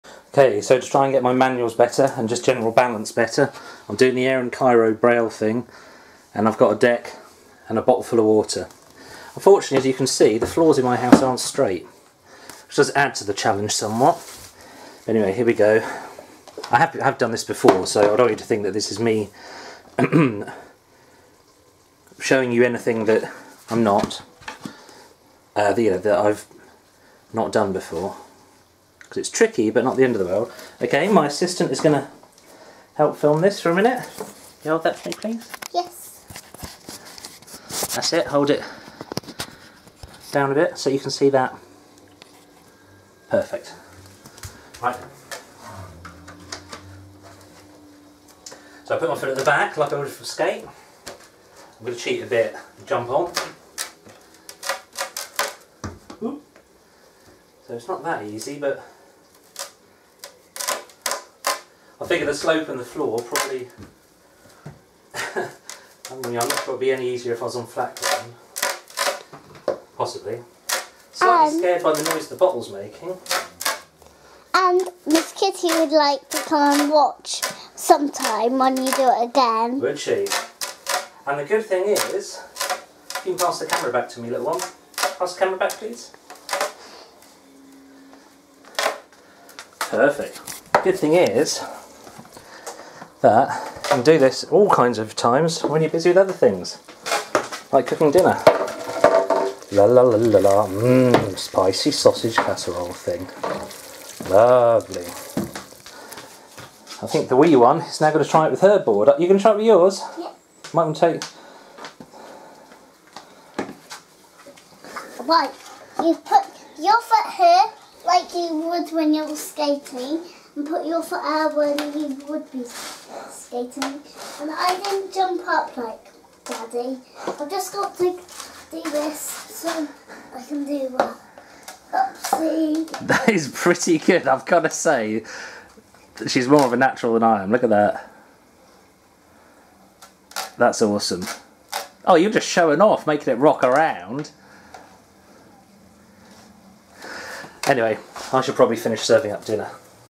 Grey Dad Skater Progress 26.5 - Balance Board Edition
To try and improve my balance on my skateboard I built a 'balance board'.  I got the idea from the Braille skate channel.